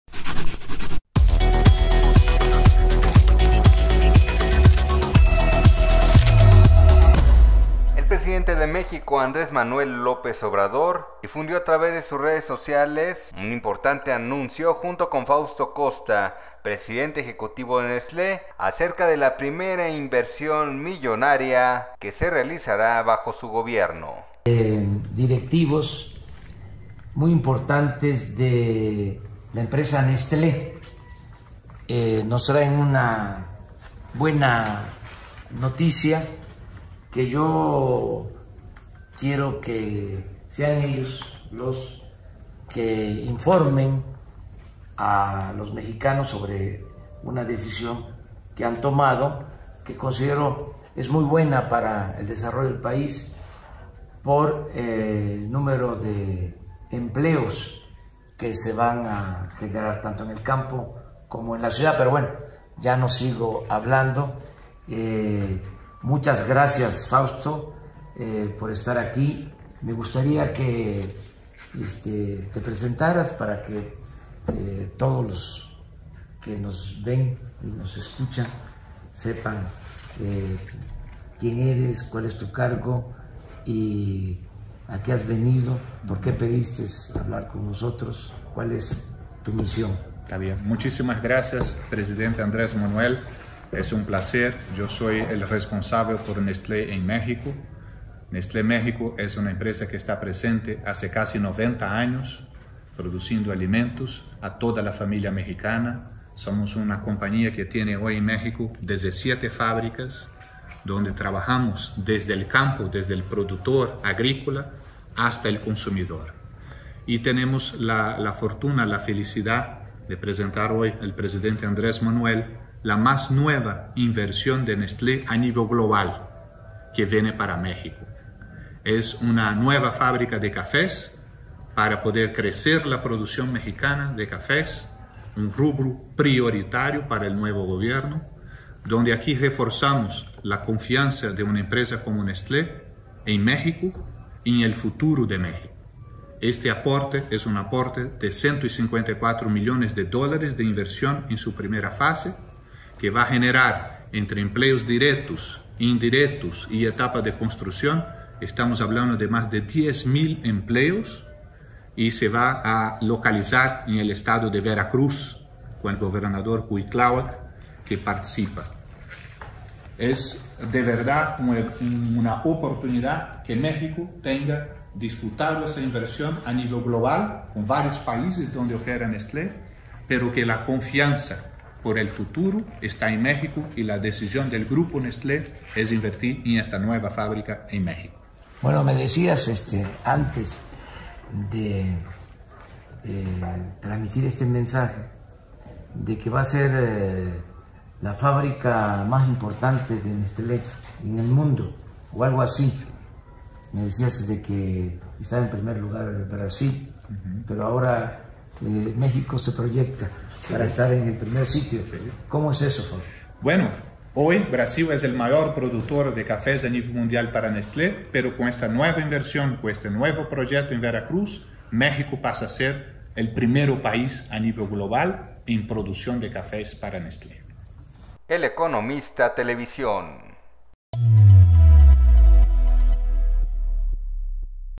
Nestlé anuncia a AMLO inversión millonaria en Veracruz
Fausto Costa, presidente ejecutivo de Nestlé se reunió con el Presidente de México Andrés Manuel López Obrador donde le informó que en Veracruz construirán una planta para procesar 20 mil toneladas de café, con una inversión inicial de 154 millones de dólares.